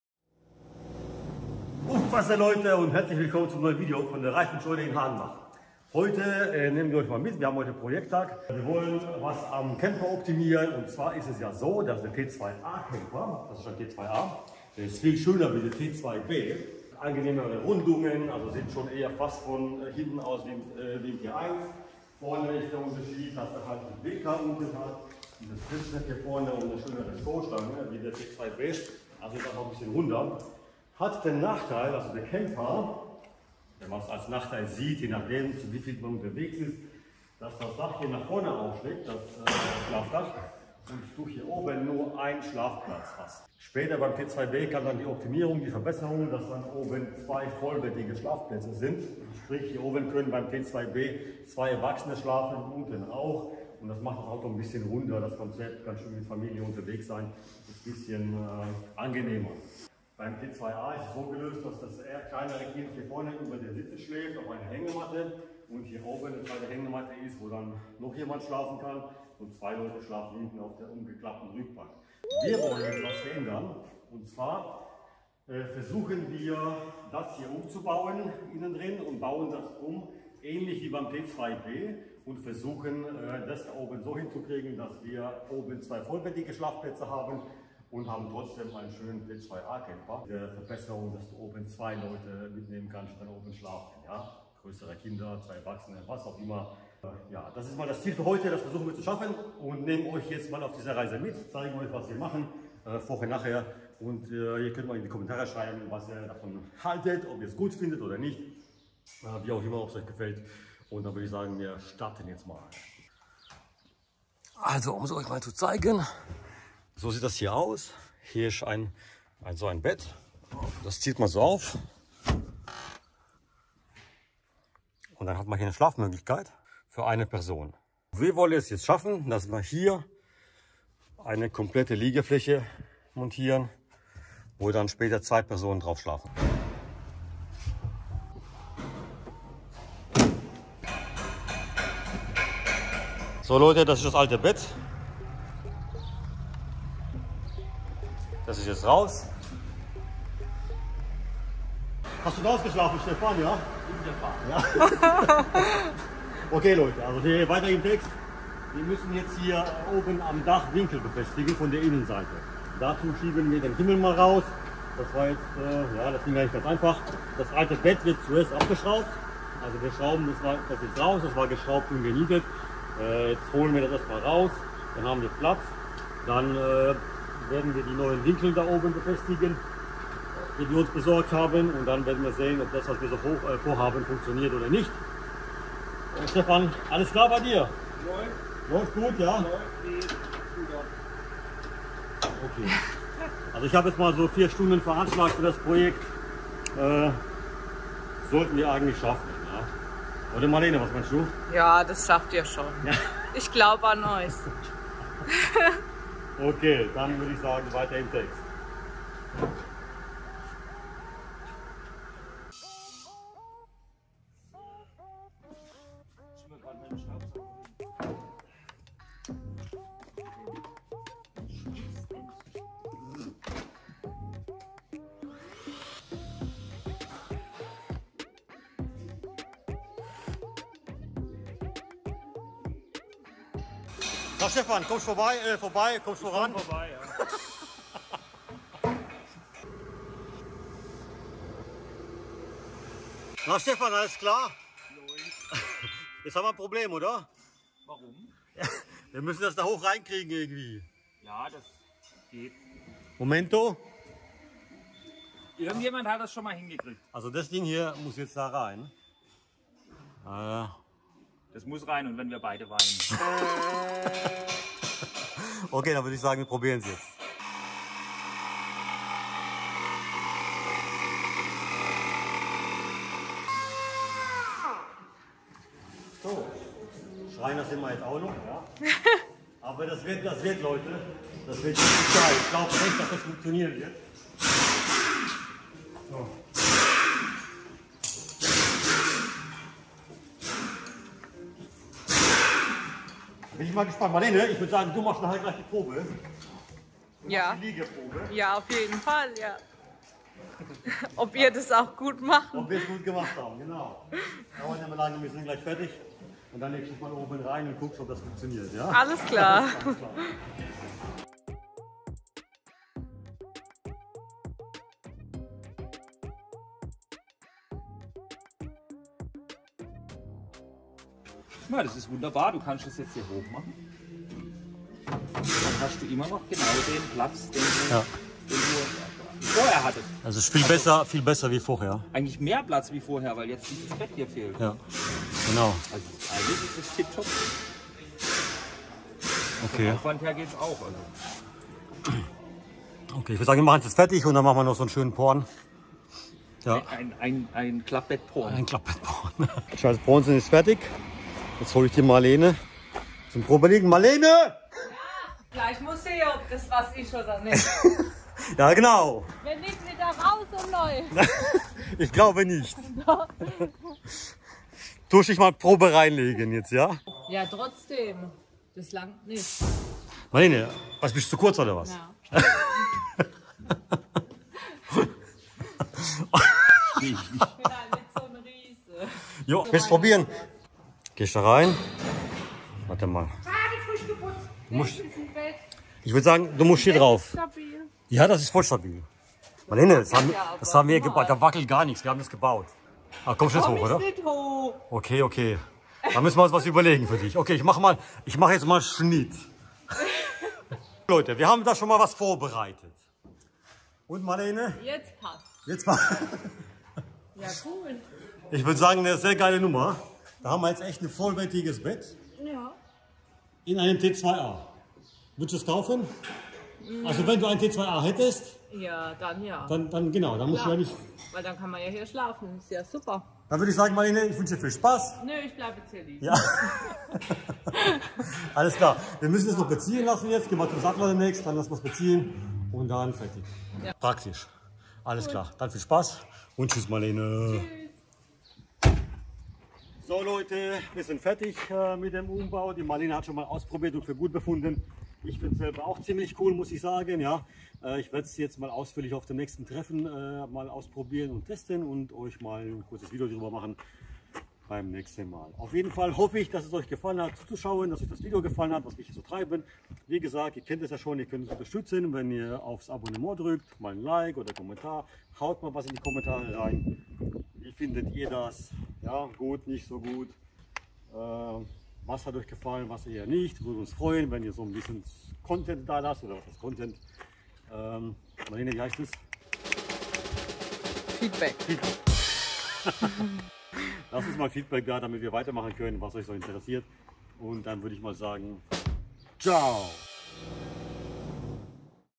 Wir bauen ein Luxusbett in unseren T2A Camper I Vanlife I Reifenscheune
Wir bauen ein Doppelbett im Hubdach unseres T2A Westfalia Campers ein.
So können jetzt statt zwei, vier Personen drin schlafen.